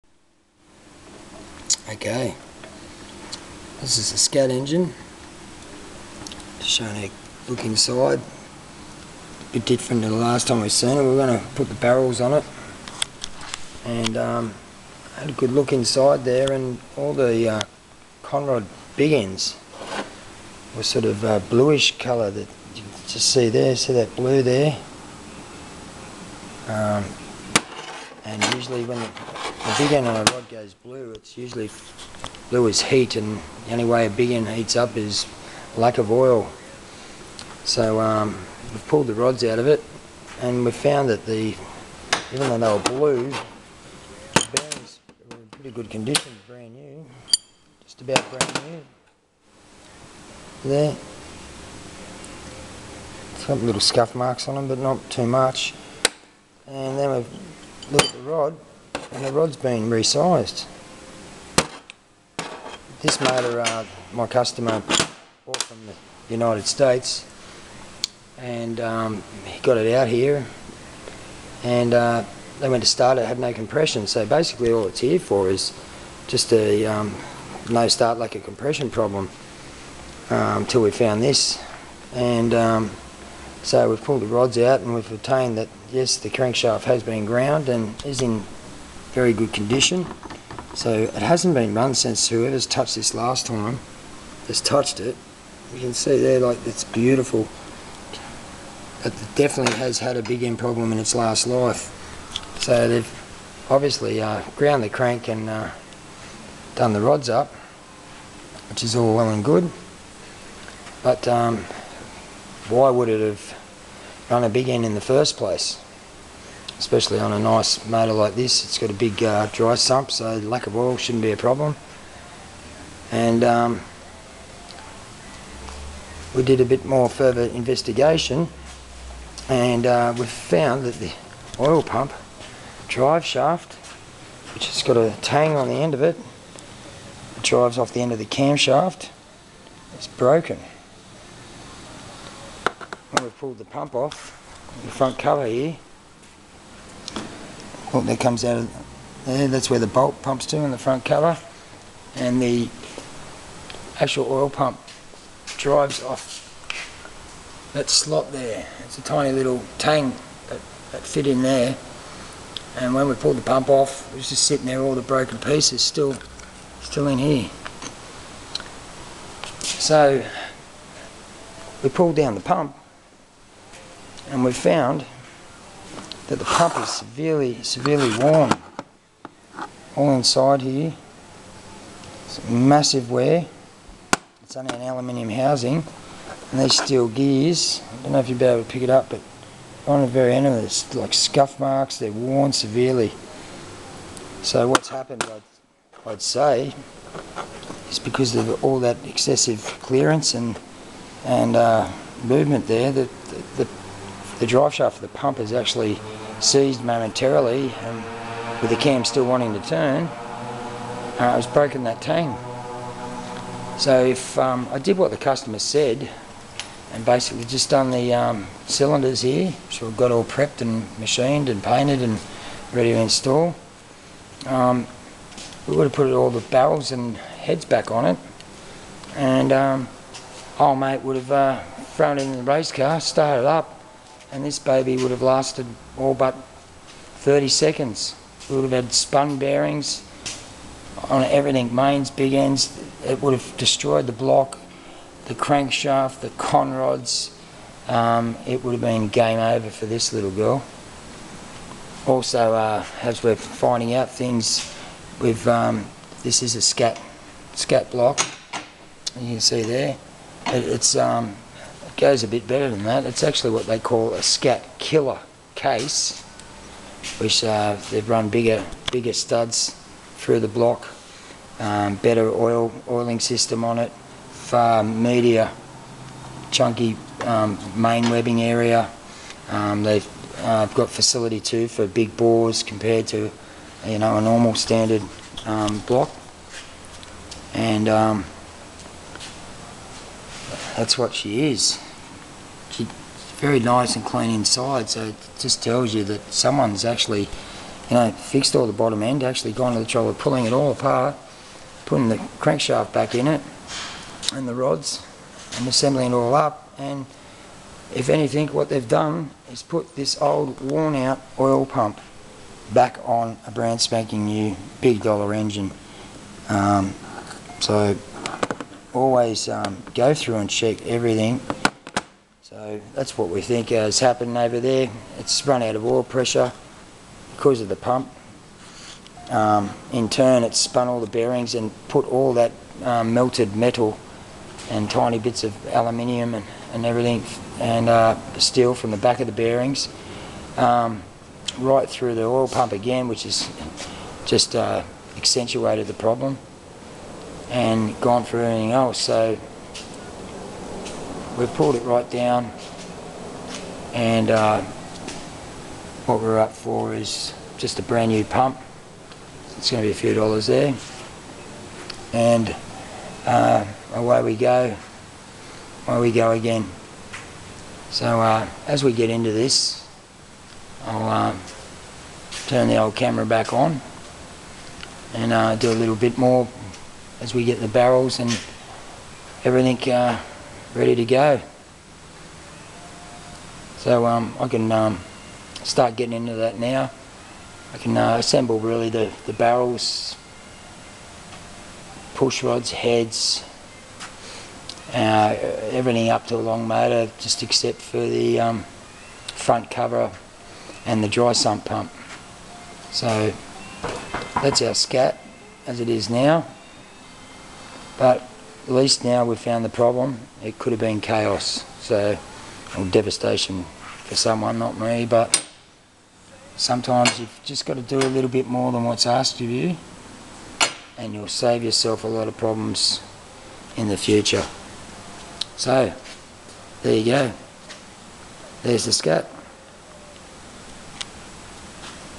Problem solving Hi Peformance Scat Killer Case Race Engine failures
A very close look at a Scat Killer Case methanol injected speedcar engine,fitted some tough internals.